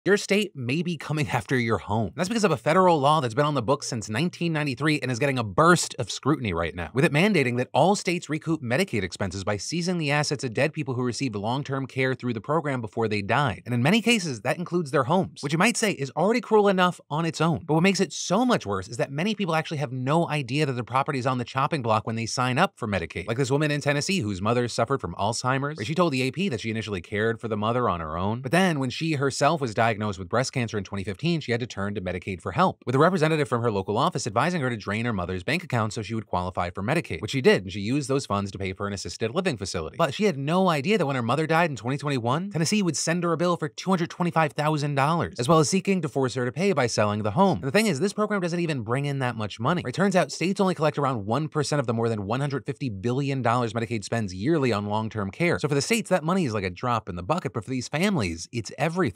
On Medicaid? The Government Might Steal Your House
Art Department: William Crespo
Writing/Research: Chris Tolve, Philip DeFranco